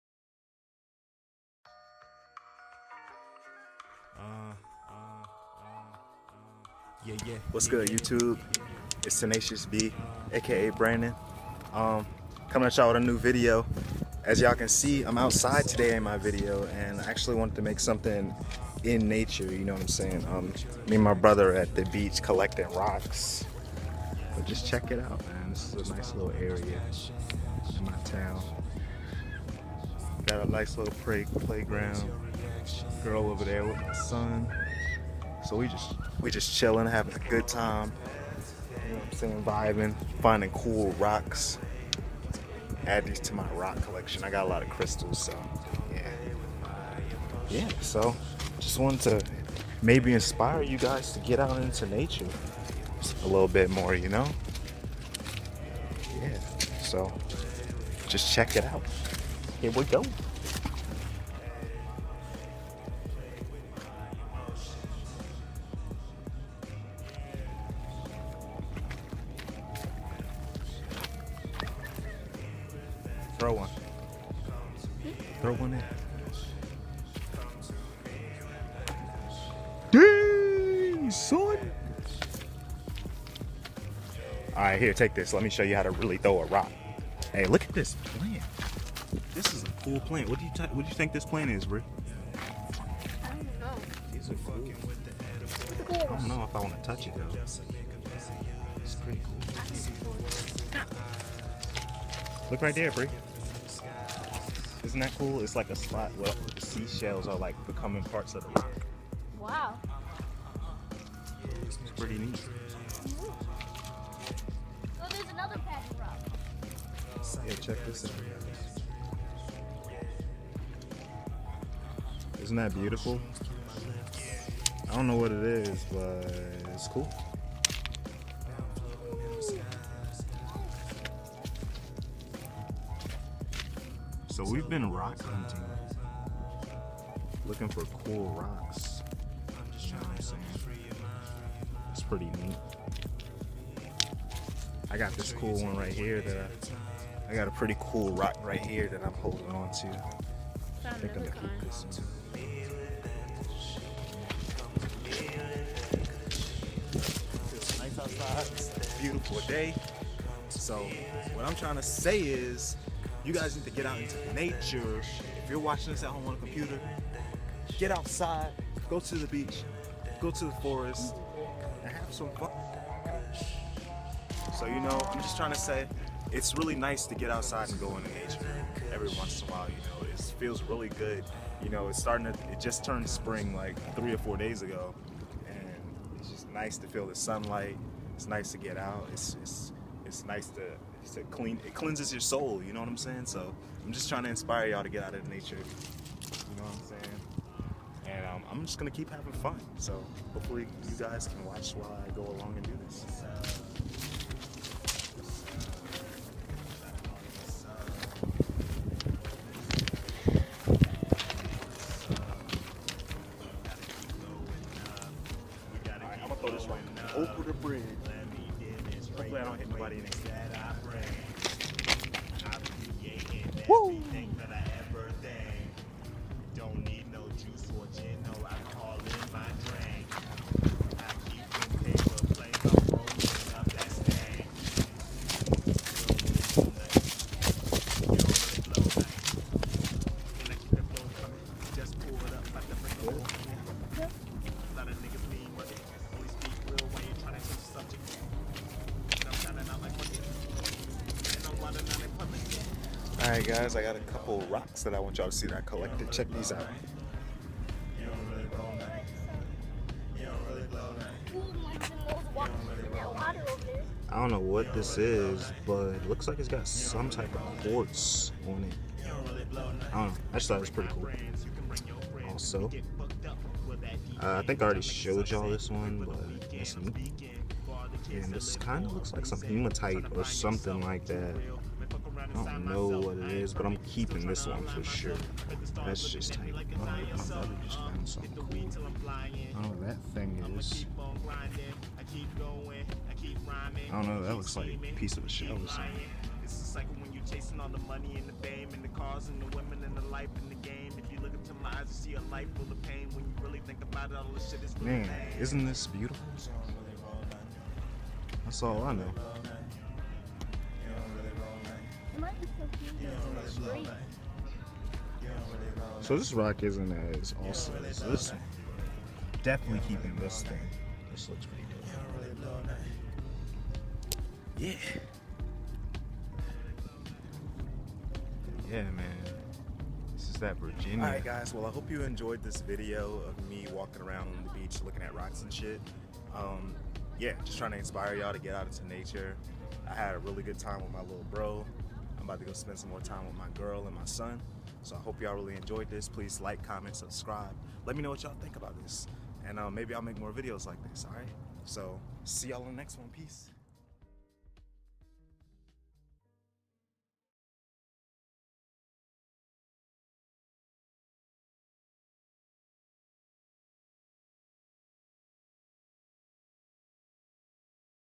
Explore Nature!!! (James River edition)
Exploring by the river with my brother!